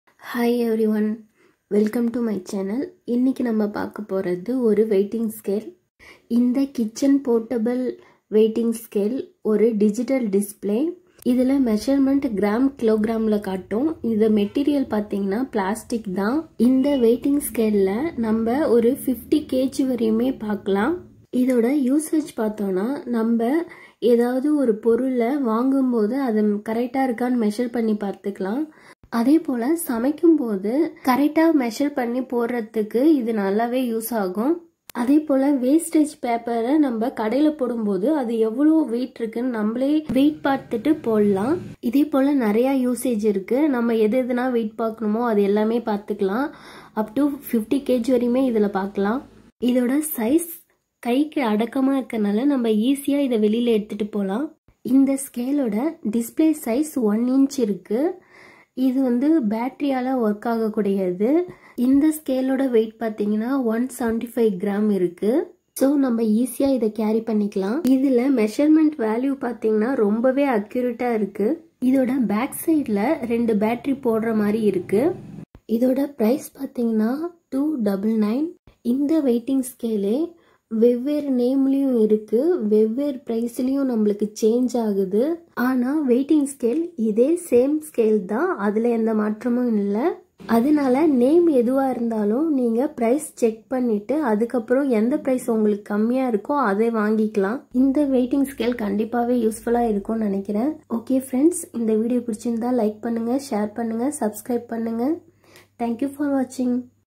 WeiHeng Kitchen portable Weighing Scale 💙/ Digital Electronics Scale 💯/ Review in Tamil
Kitchen portable premium WeiHeng Weighing Scale full review in tamil.
🔥 Measure weight maximum upto 50 Kg
🔥 Measures in Gram & Kilogram
🔥 Accurate Measurements
🔥 low price

buy 🛍 product click 👇👇below Link :